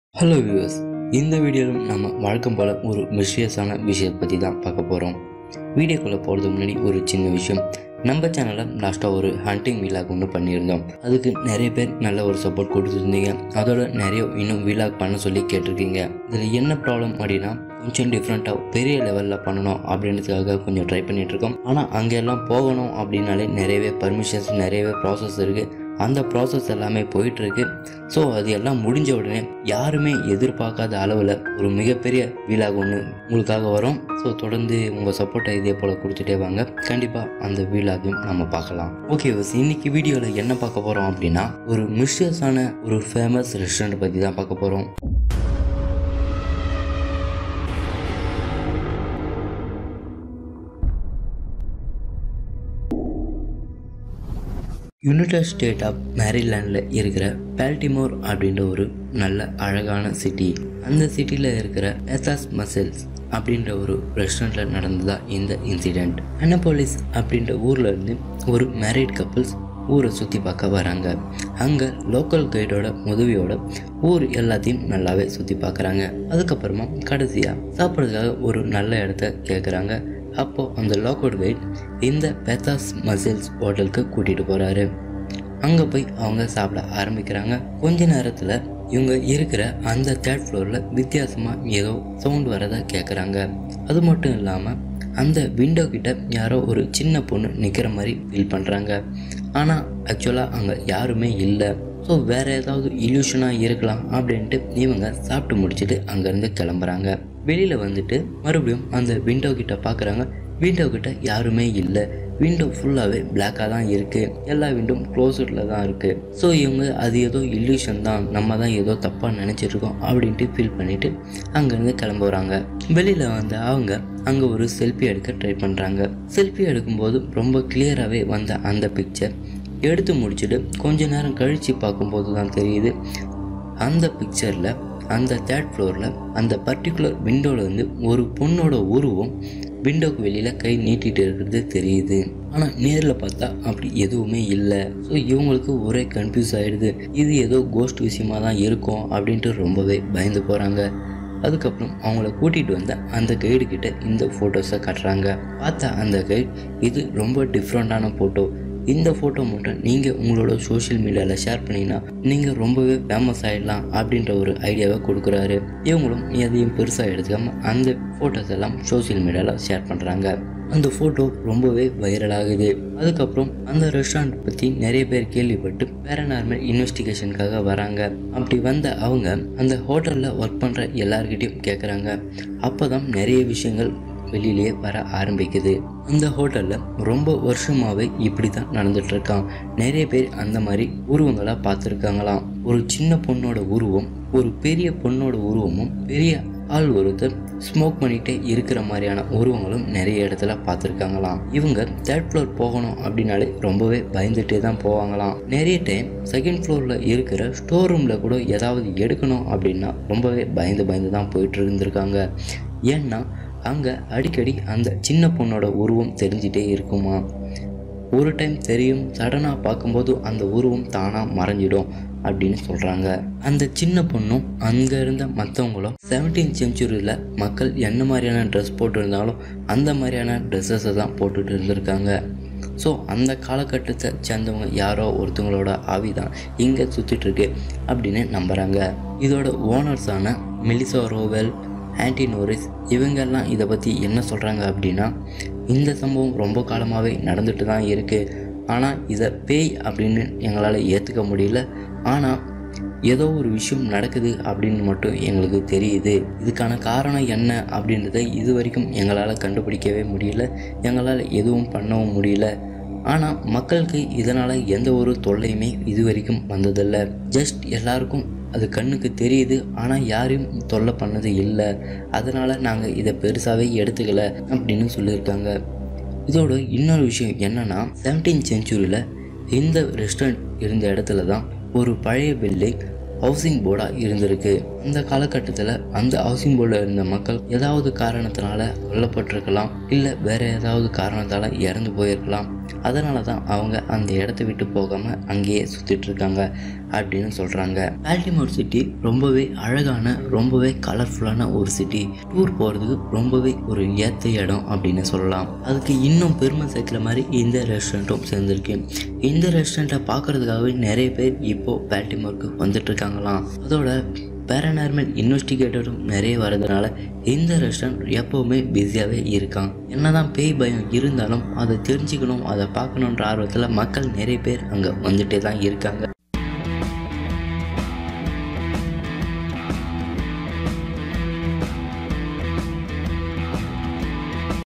Real Ghost Caught On Camera Footage /Tamil Video /Ghost Found In Tape/ திகிலான ஜன்னலோர பேய் வீடியோ
WELCOME 2 Real Tamil Horror
 A spooky sight was captured on camera in Baltimore over the weekend. While on a ghost tour, a Maryland couple took a picture of what looks like a ghost on the third floor of Bertha’s Mussels in Fell’s Point.

Baltimore Ghost Tours posted the photo from the Annapolis couple on Monday. They believe it’s the ghost of a little girl

he photo was taken Saturday evening sometime between 7 p.m. and 9 p.m. along Lancaster Street.

Nicole Henry, of Annapolis, said she was stunned by what her husband, Joseph, captured. She said Bertha’s was one of the first stops on the haunted pub tour and after they left the bar he husband decided to snap a picture of the outside of the building.

video பாத்துட்டு
உங்களோட கருத்துகளை மறக்காம பதிவு பண்ணுங்க .